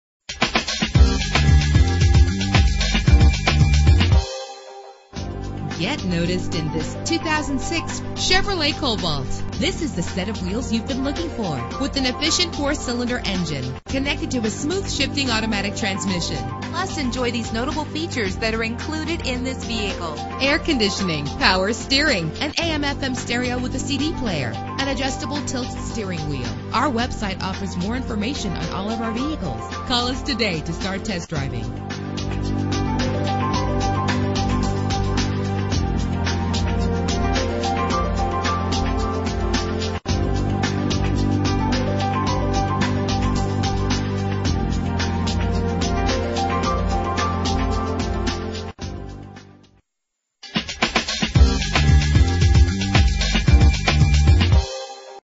2006 Chevrolet Cobalt - Manchester NH
Year: 2006
Make: Chevrolet
Model: Cobalt
Trim: LS
Engine: 2.2 liter inline 4 cylinder 16 valve
Transmission: Automatic
Color: Blue
Mileage: 93112
Address:
1477 S Willow St